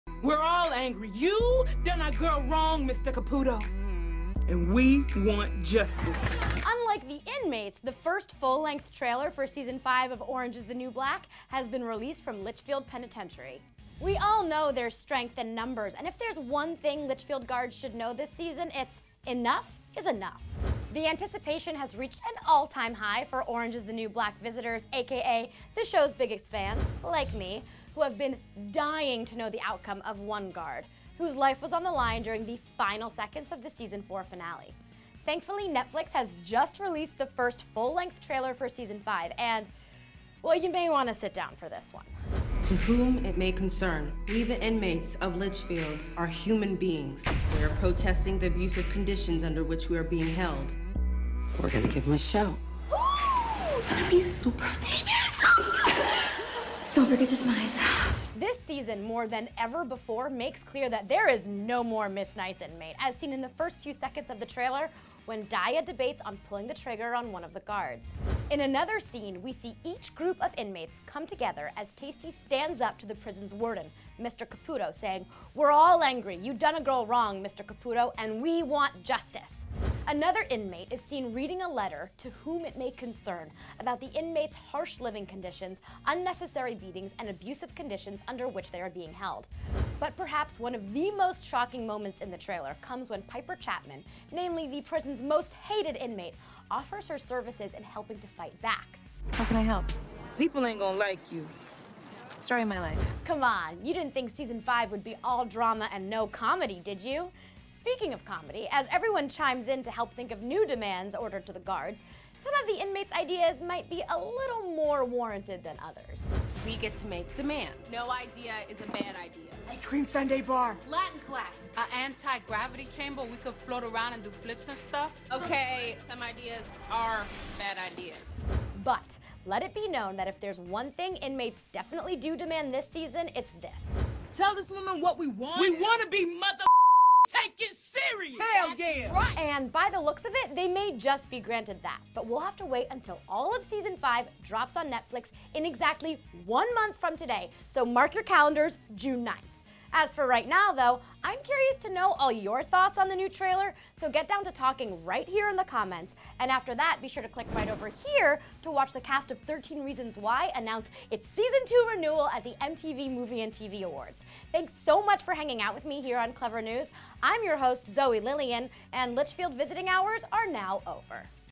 Orange Is The New Black Debuts INTENSE Season 5 Trailer
The first full-length trailer for season 5 of Orange is the New Black has been released from Litchfield Penitentiary, unlike the inmates, who are fighting for their rights in what could only be described as powerful retaliation.
We all know that there’s strength in numbers, and if there’s one thing the Litchfield guards should know this season – enough is enough.

The anticipation has reached an all-time high for Orange is the New Black visitors AKA the show’s biggest fans, like myself, who have been dying to know the outcome of one guard, whose life was on the line during the final seconds of the season 4 finale. Thankfully, Netflix has just released the first full-length trailer for season 5, and, well, you may wanna sit down for this one…
This season more than ever before makes clear that there is NO more miss nice inmate, as seen in the first few seconds of the trailer when Daya debates on pulling the trigger on one of the guards. In another scene, we see each group of inmate come together as Taystee stands up to the prison’s warden, Mr. Caputo, saying “We’re all angry! You done a girl wrong, Mr. Caputo, and we want justice!”

Another inmate is seen writing a letter to ‘whom it may concern’ about the inmates’ harsh living conditions, unnecessary beatings, and abusive conditions under which they are being held. But perhaps one of the most shocking moments in the trailer comes when Piper Chapman, namely the prison’s most hated inmate, offered her services in helping to fight back…
Come on, you didn’t think season 5 would be all drama and NO comedy now, did you?! Speaking of comedy, as everyone chimes into help think of new demands ordered to the guards, some of the inmates’ ideas may have been more warranted than others…
BUT let it be known that if there’s one thing the inmates definitely DO demand this season, it’s this:
And by the looks of it, they may just be granted that, but we’ll have to wait until season 5 in its entirety drops on Netflix in exactly one month from today on June 9th so mark your calendars. As for right now, I’m curious to know all your thoughts on the new trailer, so get to talking right down here in the comments, and after that, be sure to click right over here to watch the cast of 13 Reasons Why announce its season 2 renewal at the MTV Movie & TV Awards. Thanks so much for hanging out with me here on CLevver News, I’m your host Zoe Lillian and Litchfield visiting hours are now OVER.